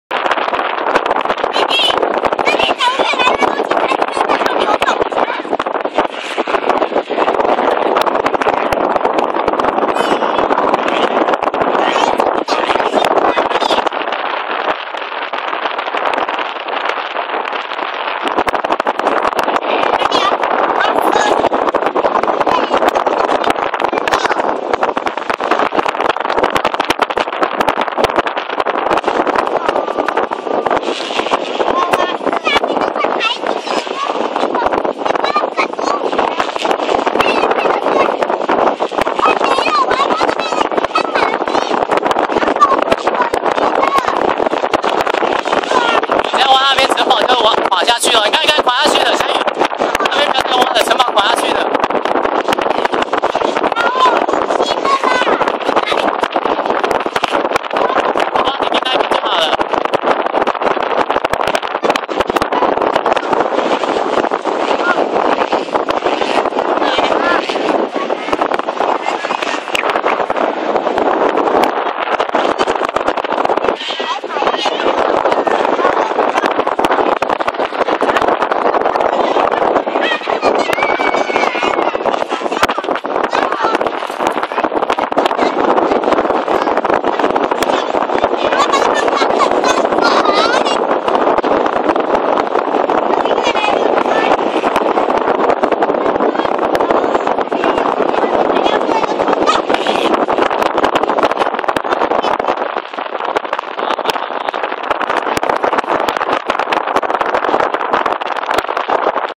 2012_07_14_ 新屋綠色走廊海邊玩砂
天氣太熱了, 一到傍晚, 海邊砂灘就擠滿了玩水的人潮~